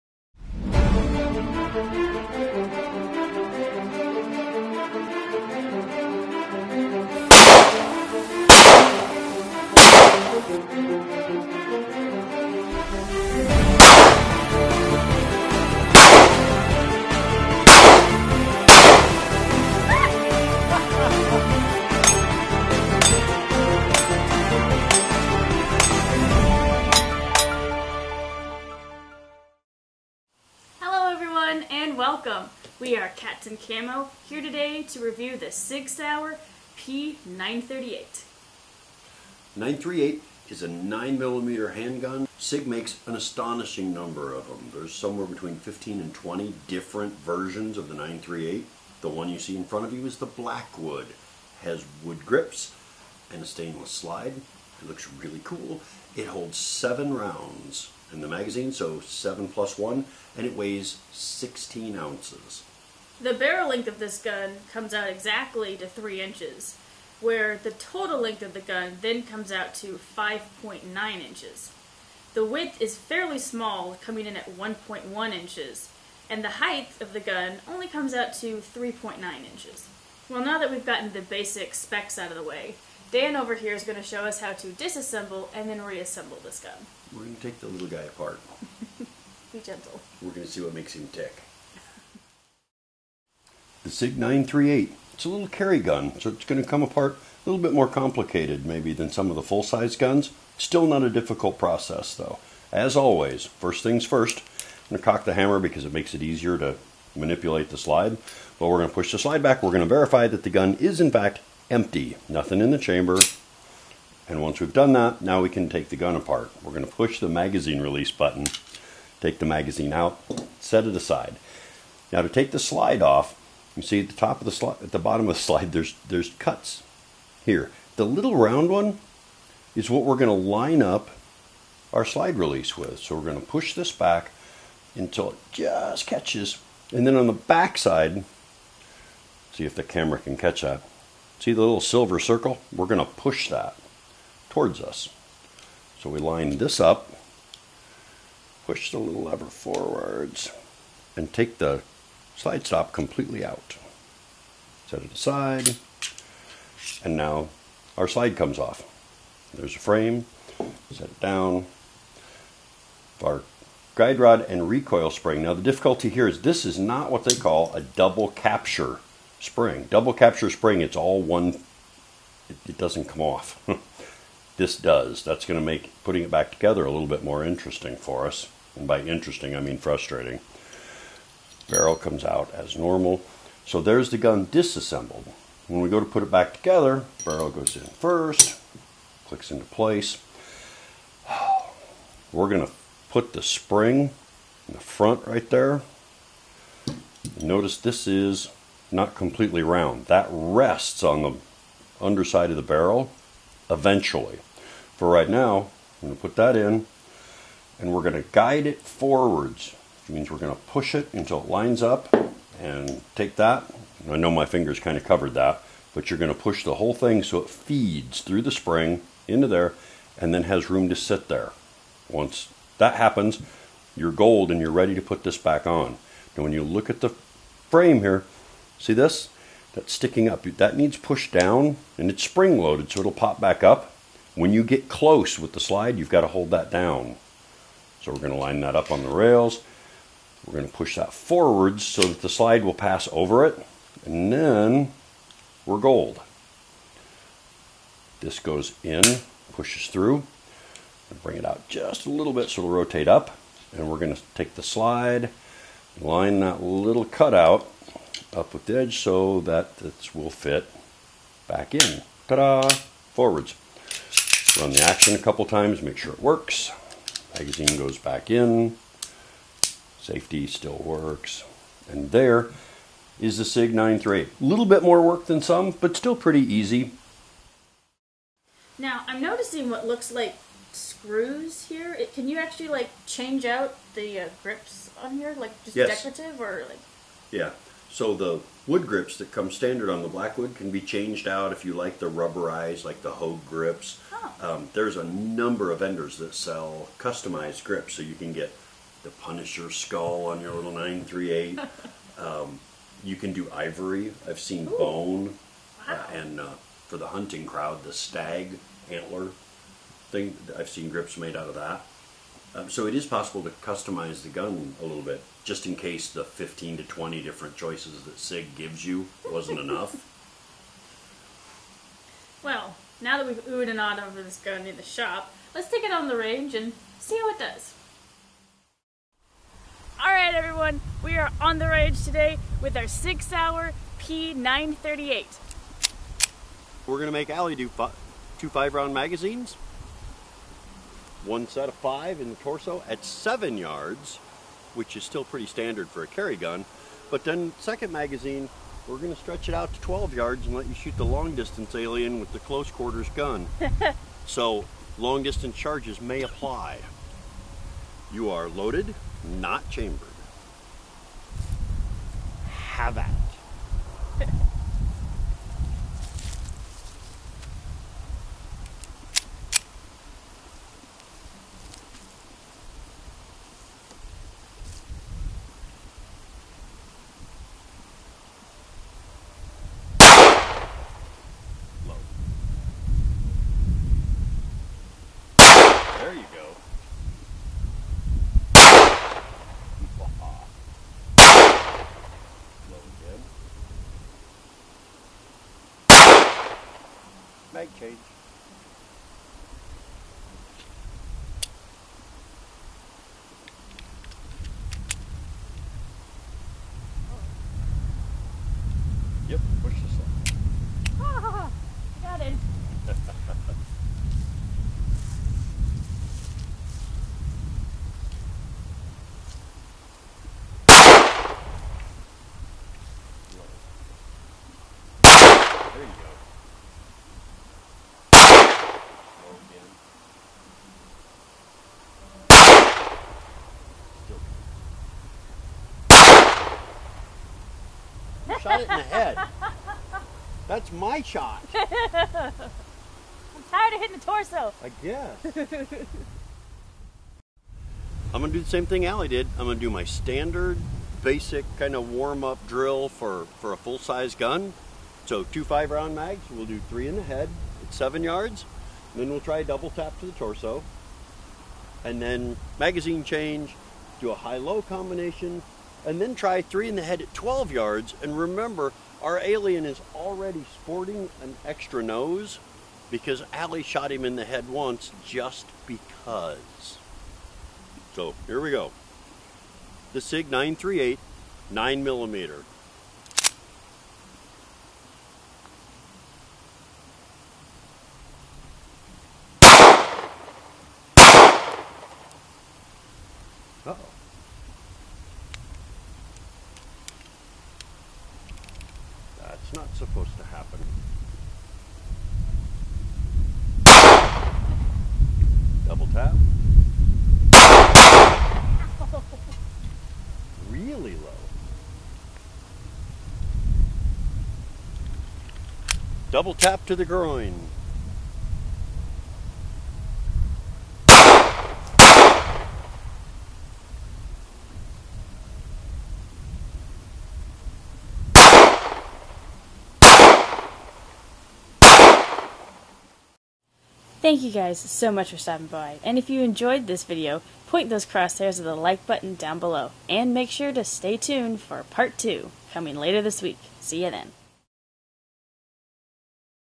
Let's shoot the Sig Sauer P938
In part one we give you the basic specks, show how to disassemble and reassemble this gun as well as doing some basic shooting.